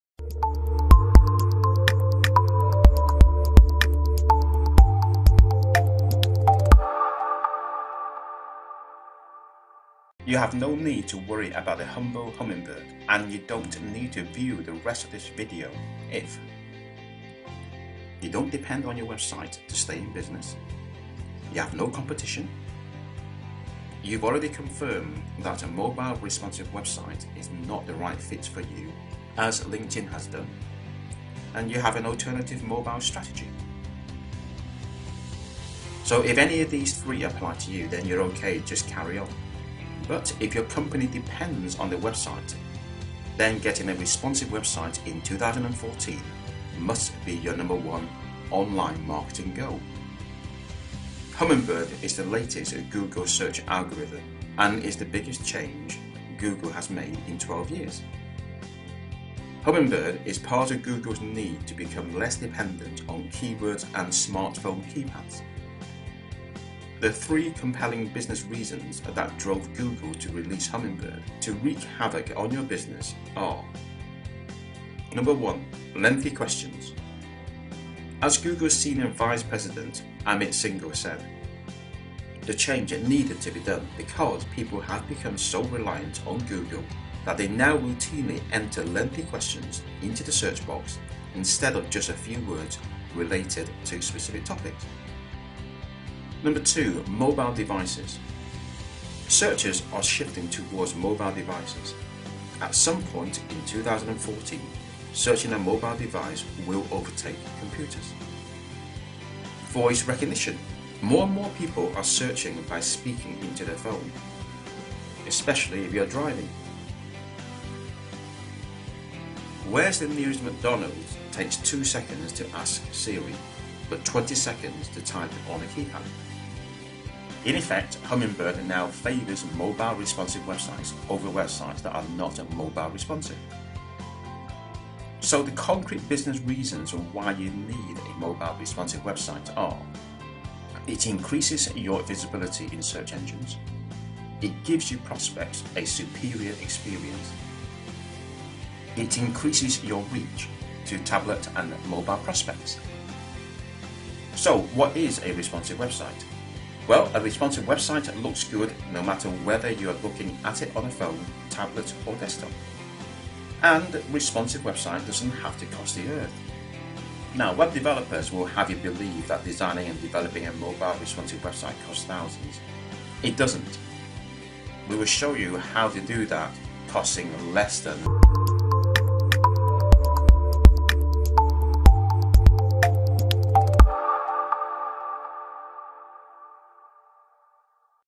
Responsive Website Design - Google Hummingbird Update - Cyber Knight Limited
Google Hummingbird Update algorithm is designed to target Conversational Searches commonly performed on Mobiles, Tabs Voice Search and Google Chrome's Web Browser. So you have to prepare your website mobile devices ready or mobile responsive website.